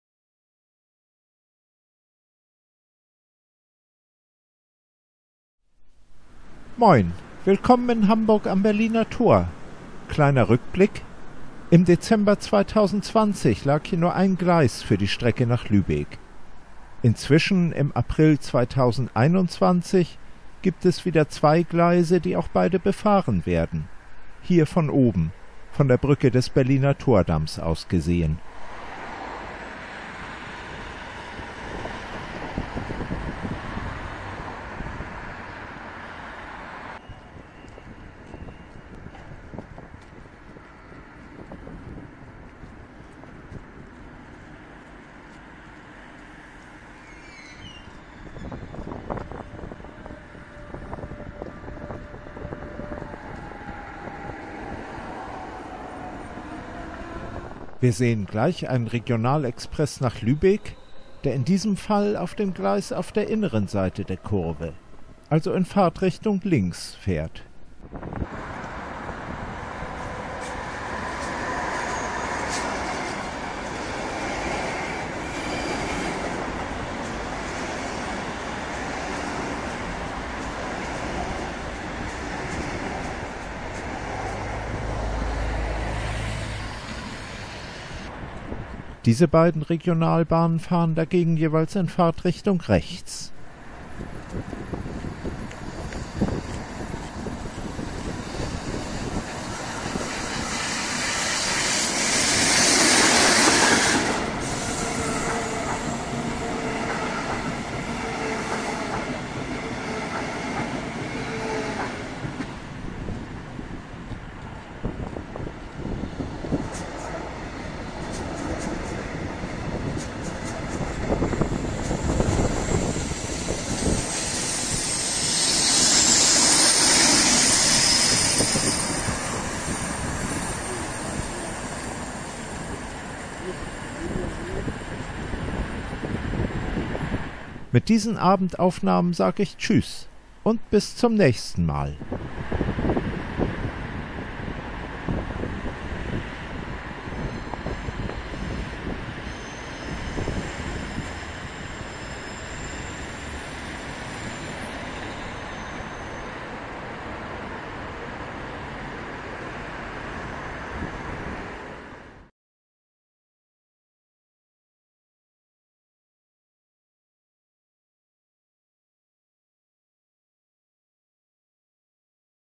S-Bahn und Regionalbahn in Hamburg: Bauarbeiten am Berliner Tor – Ansichten vom April 2021
English subtitles available – Berliner Tor ist ein wichtiger Knotenpunkt im Hamburger S-Bahn-Netz. Außer den S-Bahnen fahren hier die Züge nach Berlin und Lübeck ohne Halt durch. Im Dezember 2020 lag hier nur ein Gleis für die Strecke nach Lübeck. Inzwischen, im April 2021, gibt es wieder zwei Gleise, die auch beide befahren werden. Im Video ist ein Regionalexpress nach Lübeck zu sehen, der in diesem Fall auf dem Gleis auf der inneren Seite der Kurve, also in Fahrtrichtung links, fährt. Es folgen zwei Regionalbahnen, die jeweils in Fahrtrichtung rechts fahren.

Berliner Tor is an important junction in the suburban rail network of Hamburg, Germany. Not only suburban trains serve this station, but other trains towards Berlin and Lübeck pass by non-stop. In December, 2020, there was only one track for trains to Lübeck. Now in April, 2021, there are two tracks again, and both of them are used. In this video a regional express to Lübeck can be seen, passing on the inner track of the curve, i. e. on the left hand side. Two regional trains follow, each of them going on the right hand side.